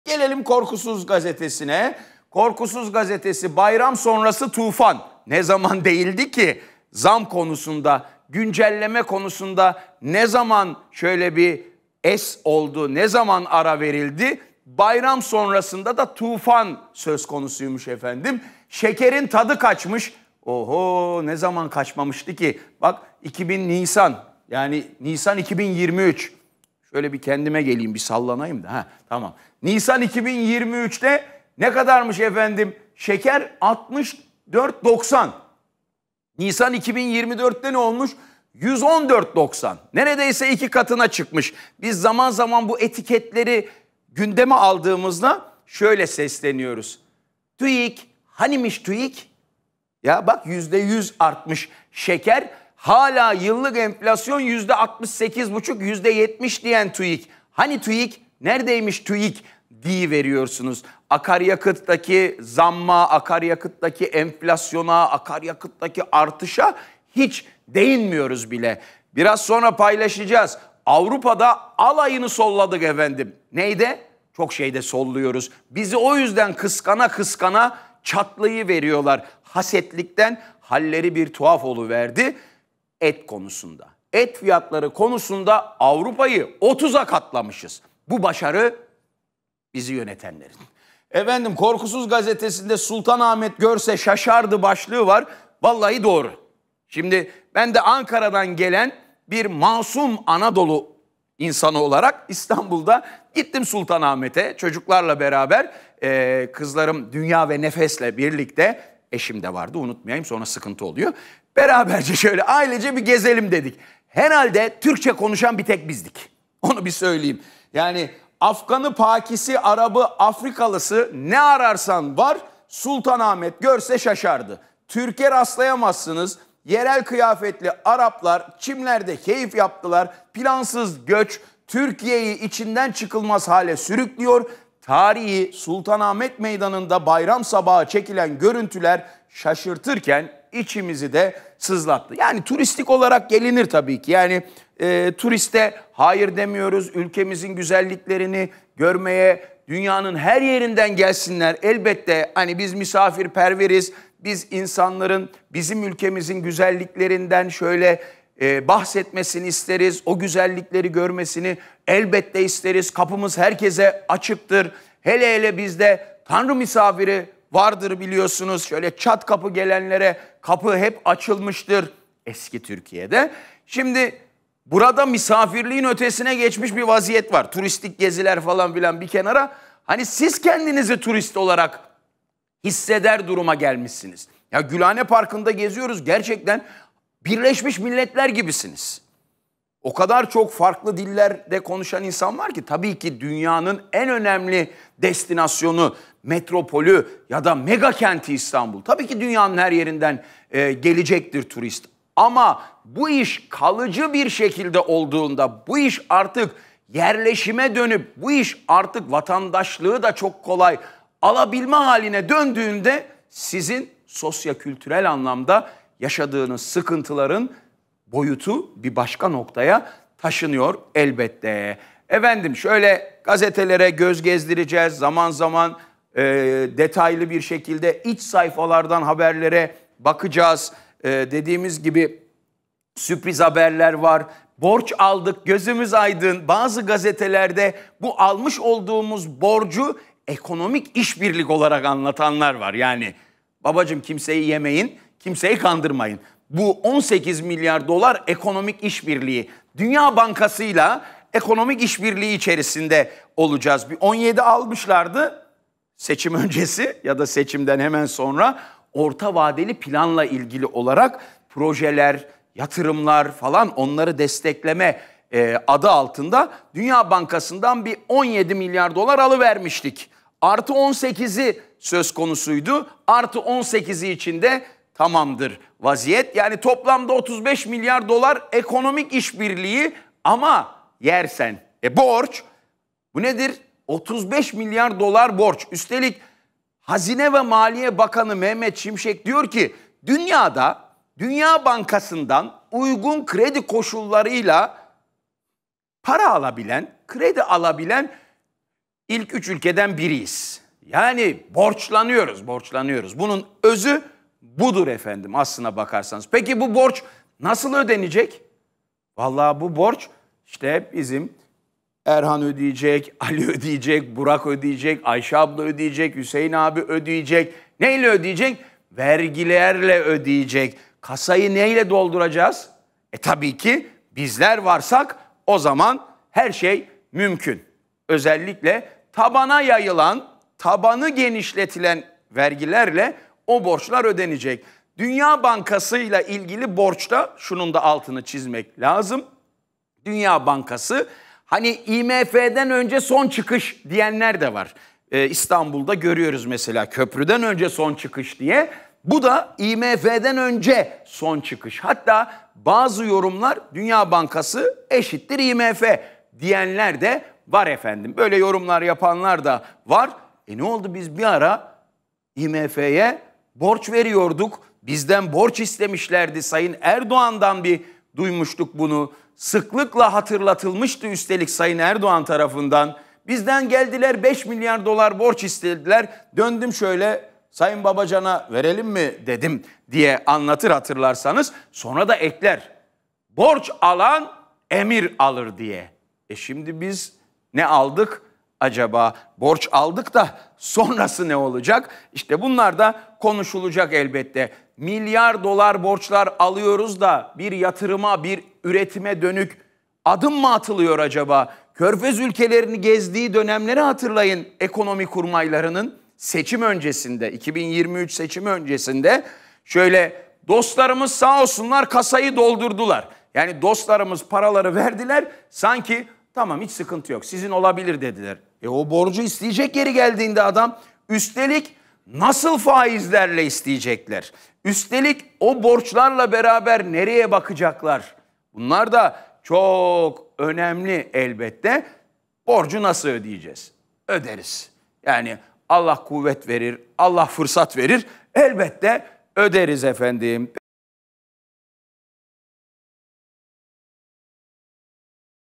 Tarihi Zamlar Yolda! Bayram Sonrası Zam Tufanı Geliyor! Savaş Kerimoğlu'ndan Çarpıcı Yorum
Digiturk: 60
D-smart: 93
Turkcell TV Plus: 69
Tivibu: 69
Türksat Kablo TV: 76